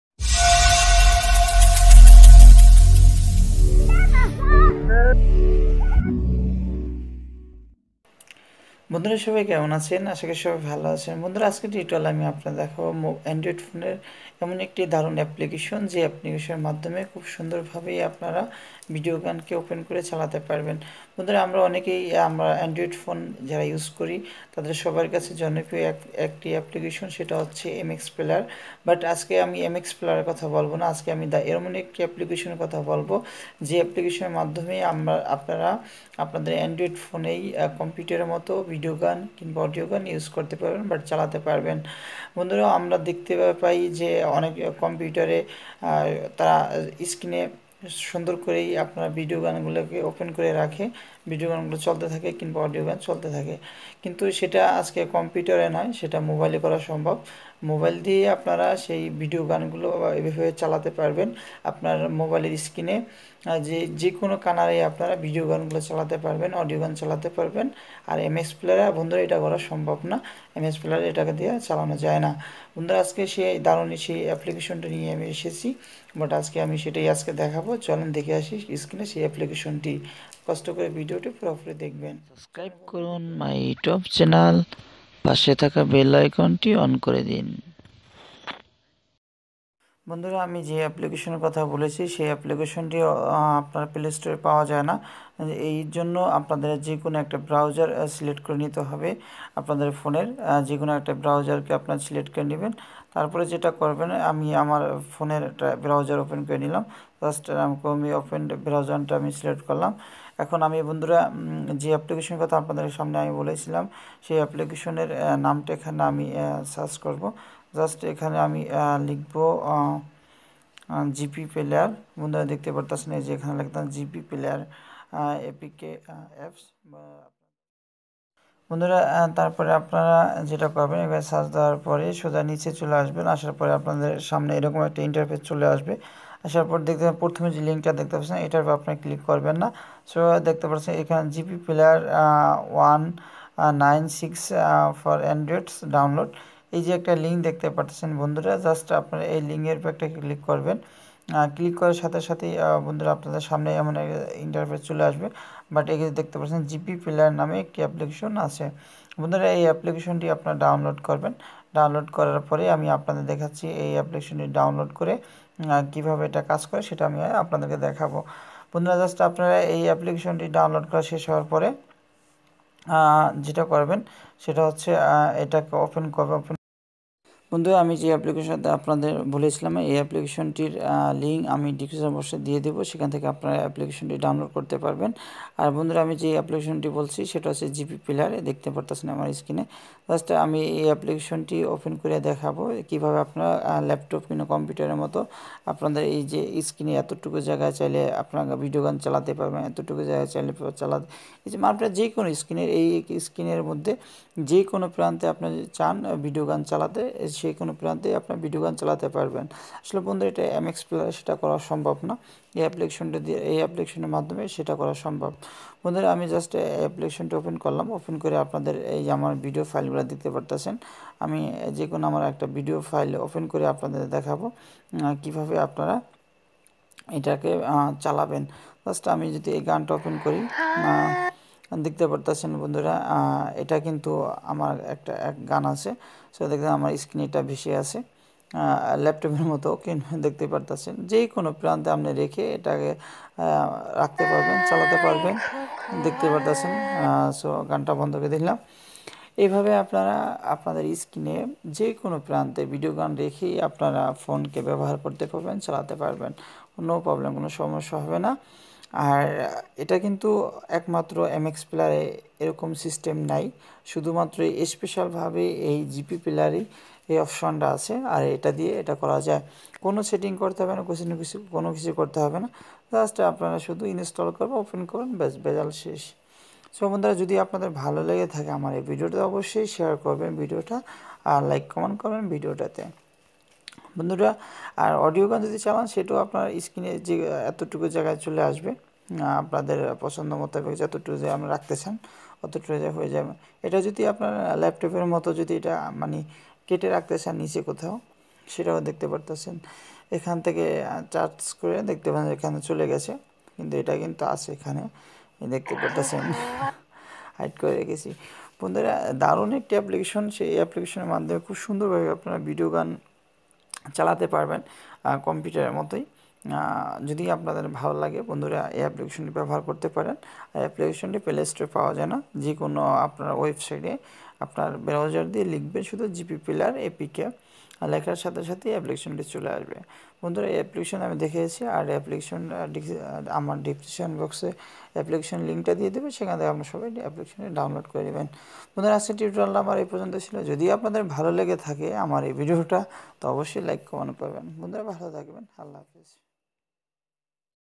কম্পিউটারের মতো মোবাইলের কিভাবে ভিডিও গান চালাবেন | How To Way Use Mobile Video Same Computer
না দেখলে মিস করবেন কম্পিউটারের মতো আপনার মোবাইলের স্ক্রিনে কিভাবে ভিডিও গান চালাবেন | How To Way Use Mobile Video Same Computer or Pc